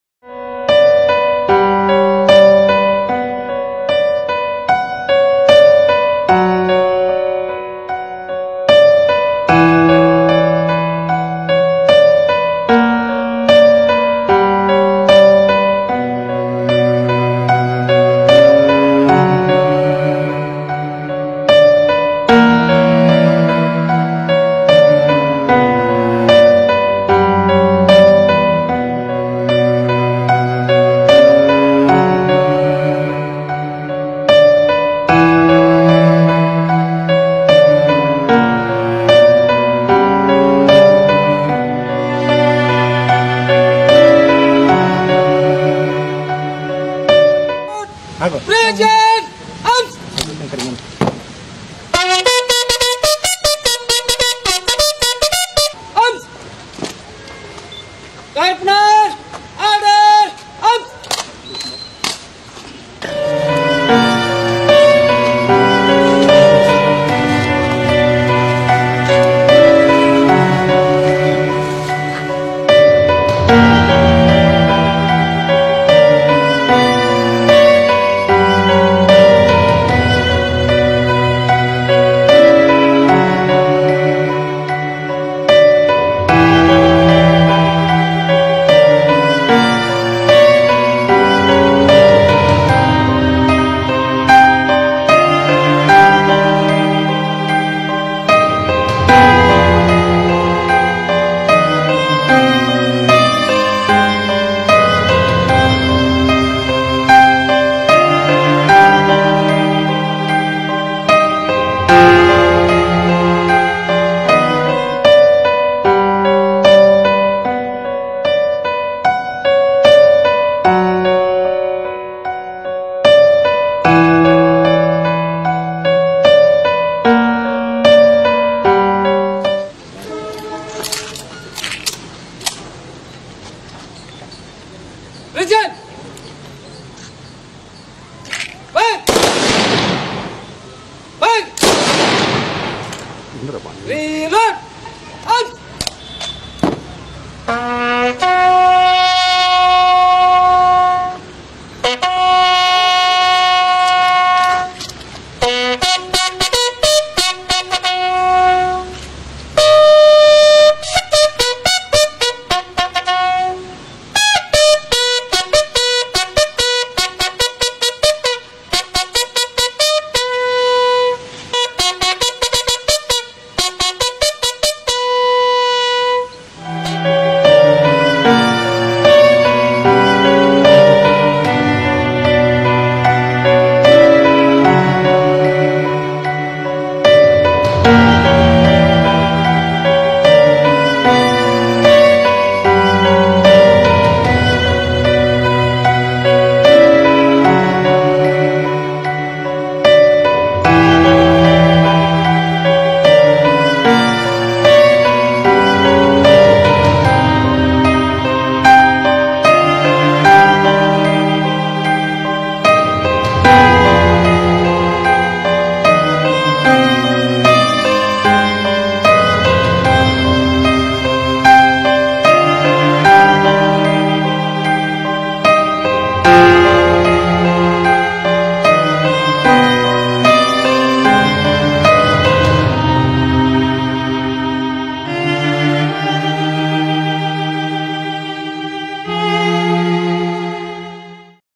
Ramya, the police dog
Ramya, the police dog gets a hero's cremation, as reported by Suvarna News, a Kannada TV channel.